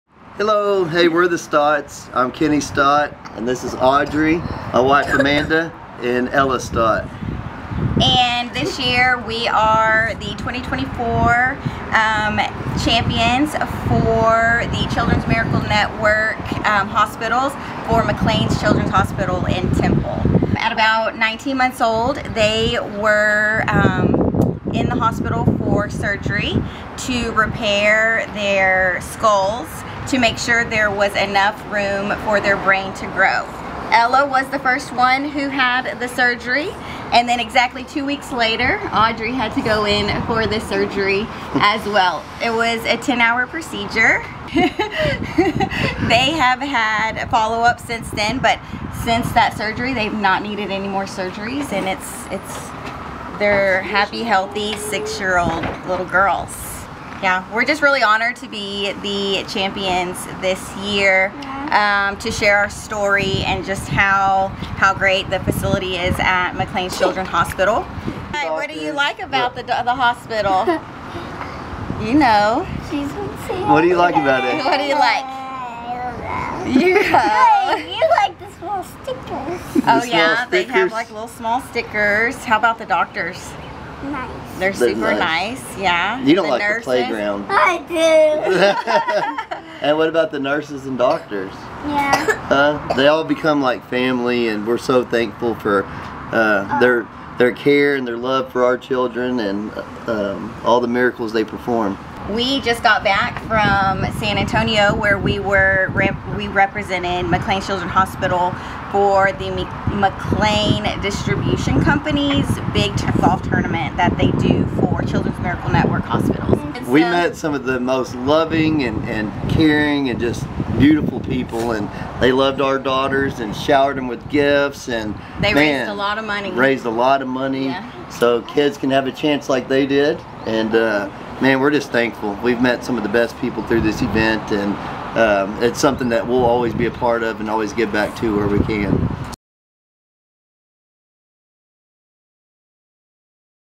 Waco-area twins become 2024 champions for Children’s Miracle Network
Waco-area identical twins Audrey and Ella Stott will be traveling across the country this year with their parents, Amanda and Kenny, to share the story of their medical journey in an effort to raise money for life-saving medical treatments, equipment and services for Children's Miracle Network Hospitals.

The Stott family is serving as the 2024 ambassadors for Children’s Miracle Network Hospitals, which serves children's hospitals in 14 counties, including McLane Children’s Medical Center in McLennan County.